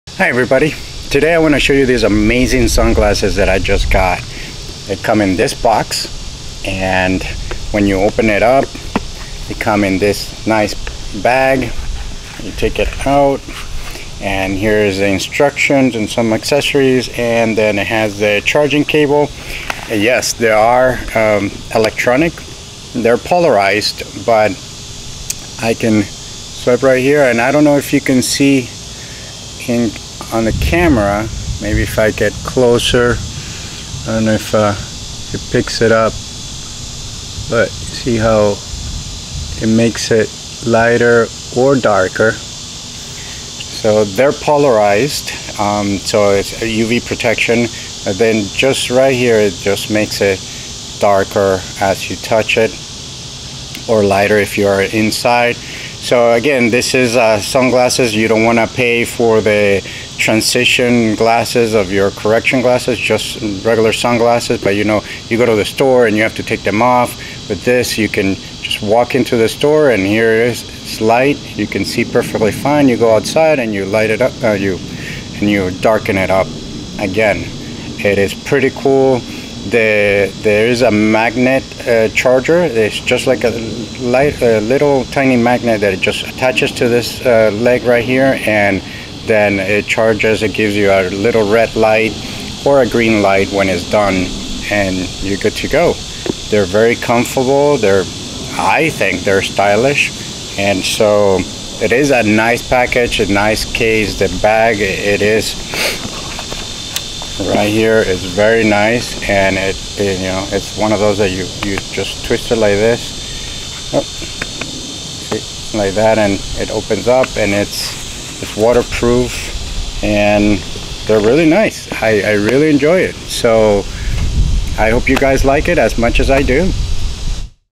Dimmable sunglasses - UV protection
The world's first dimming slider sunglasses can easily cope with various light. When in use, the transmittance of the lens can be cyclically switched by sliding the right temple back and forth. The transmittance of the lens are divided into 5 levels, which is about 7%, 16%, 23%, 31% and 37%.
The polarized sunglasses have 100% UV400 protective coating blocking 100% harmful UVA and UVB rays, which can also eliminate glare and flare, reduce eye fatigue, restore true color and protect your eyes.
Equipped with a dimming slider, the photochromic sunglasses are the most advanced patented technology. Sport sunglasses can be changed its color manually according to your remand unlike other sunglasses whose lenses change color by UV or light. The inside color of the lens is grayish-brown, which avoids visual discoloration and improve your driving safety. The color change process has a clear and natural field of vision, and the wearer can hardly feel its color change.
Photochromic sunglasses inner Nano-coating makes it possible to be splash-proof. It is perfect for sports to prevent water, such as riding, driving, mountaineering, skiing and other activities.
Magnetic charging design magnetically attaches the charger, easy to use, supports fast charging and fully charges in 30 minutes. The electronic sunglasses with a 52mAH lithium polymer battery, which can last for 47 hours on a single charge